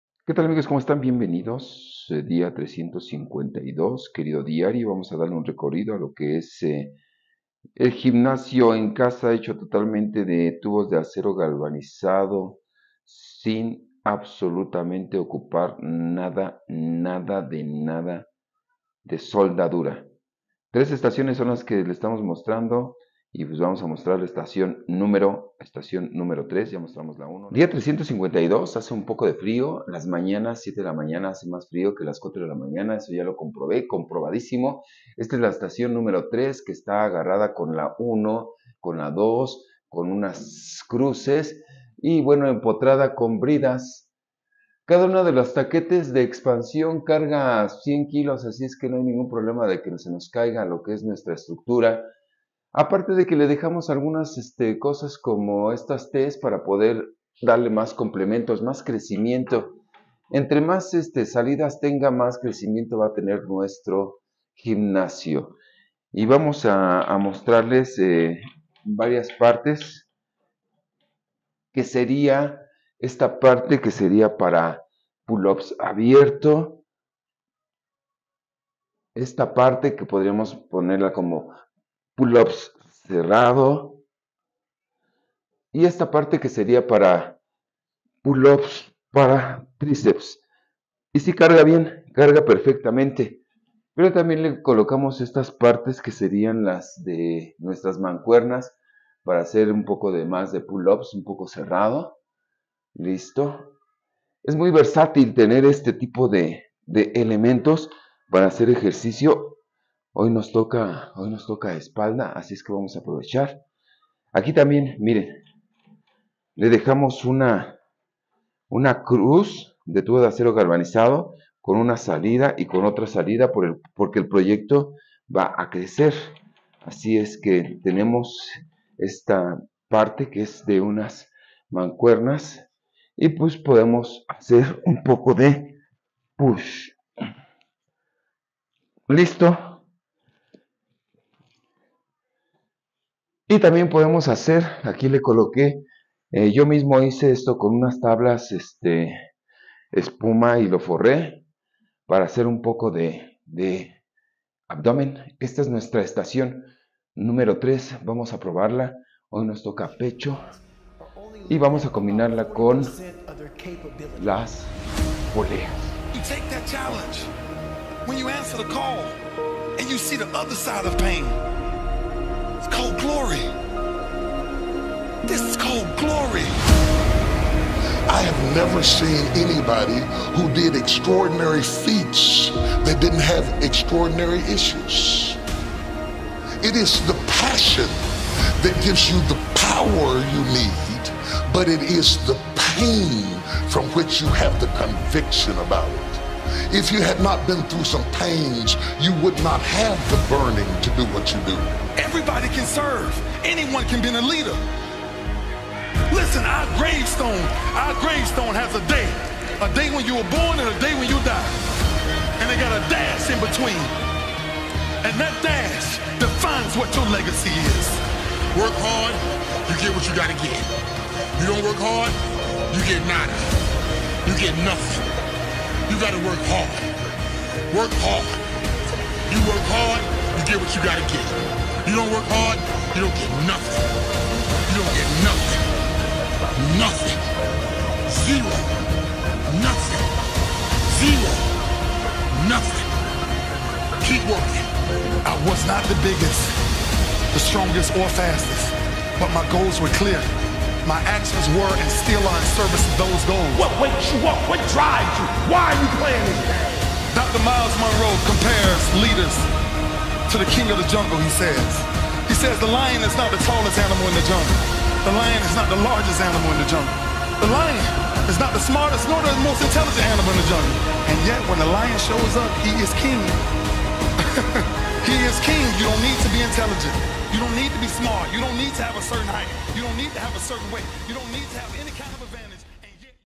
Dia 352 -Estacion 3- Objetivo: CANADA. toca espalda
En este dia les mostrare la estacion 3 de mi Gym en casa, lo probaremos con ejercicios de espalda y no olvidar porque estamos haciendo esto... OBJETIVO: CANADA